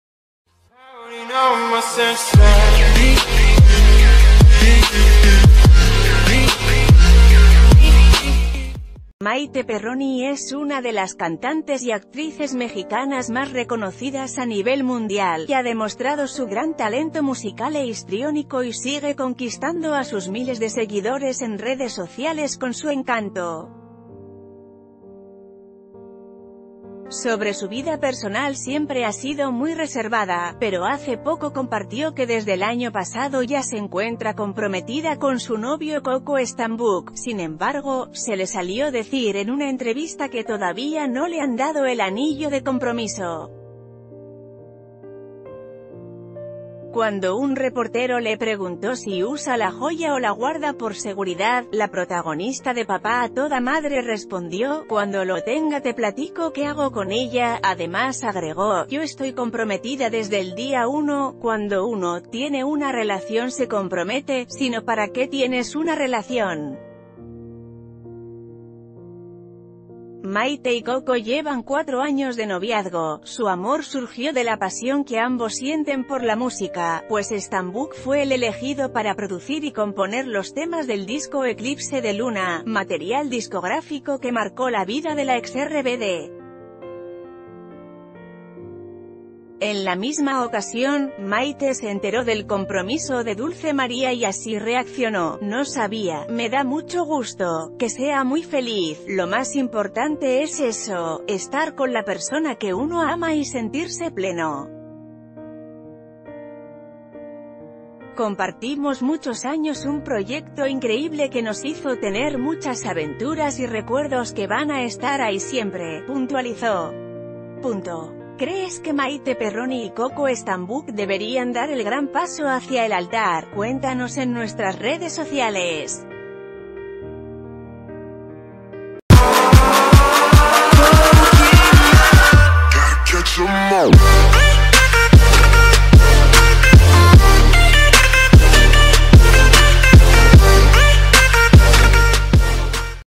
A Maite Perroni no le han dado el anillo de compromiso, ¿se quiere casar?
Maite Perroni es una de las cantantes y actrices mexicanas más reconocidas a nivel mundial, ha demostrado su gran talento musical e histriónico y sigue conquistando a sus miles de seguidores en redes sociales con su encanto. Sobre su vida personal siempre ha sido muy reservada, pero hace poco compar...